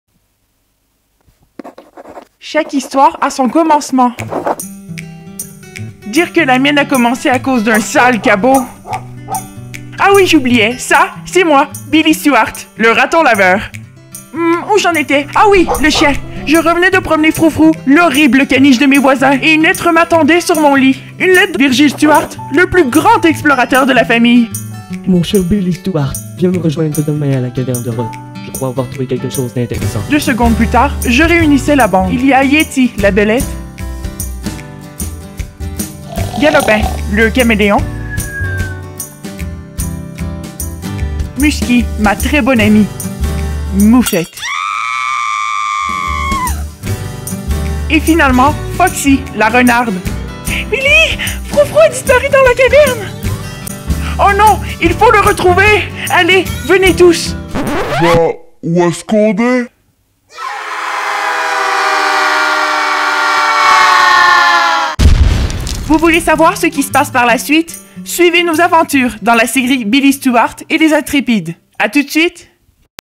Billy Stuart - Les Zintrépides
En librairie à partir du 10 Octobre
Un roman d'Alain M. Bergeron
Illustré par Sampar

Voix par Anne-Frédérique Gendron
Montage et Réalisation : Alex Bergeron